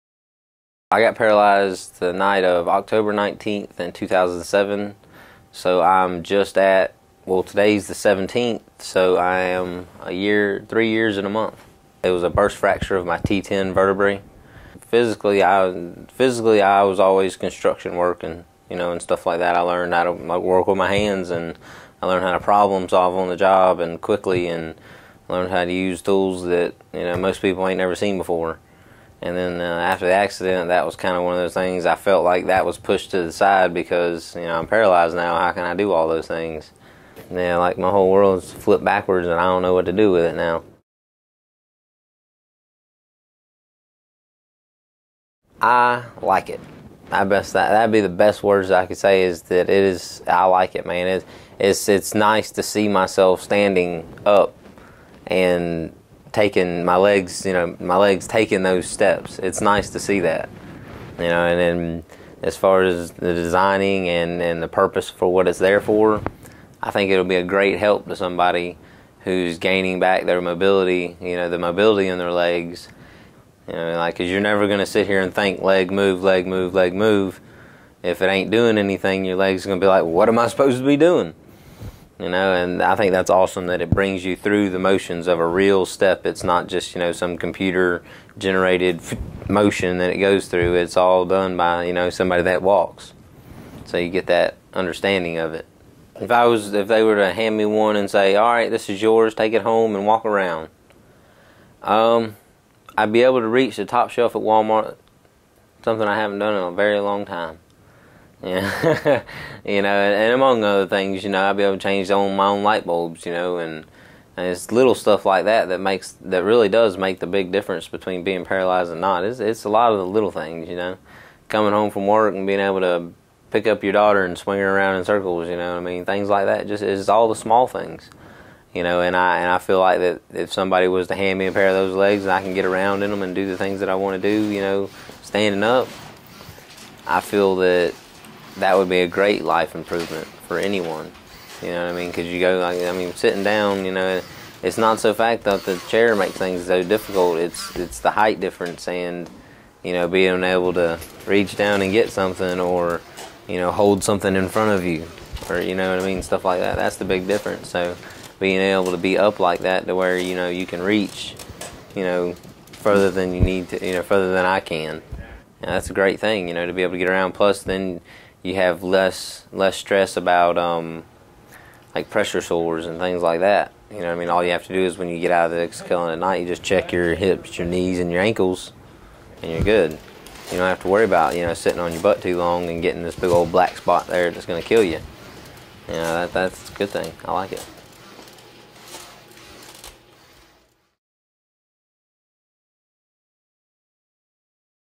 Our We Buy Houses In Connecticut president at his finest.
Jerryll our company president again making us proud. Jerryll, was not always a real estate investor. Before all the real estate adventures he embarked on, he was deeply involved with NASA robotics. We at "We Buy Houses In Connecticut" are so proud of all his accomplishments, his genuine intention to do good in the world. Here is the Exoskeleton called Mina.  I will not say anything as no words could do this justice. A paraplegic user describing it in his own words. Please enjoy and share. Sadly the leading research companies put a halt to hardware research and mostly softare is being developed because "it is cheaper". Share awareness, comment,  get involved please. Even if it is just with a simple "like". It is people like these that NEED such hardware being developed. We can't do much. Not all of us have the intelligence and know how to be such a positive influence. BUT the one thing we can do, is cause awareness. Fund hardware. Please share!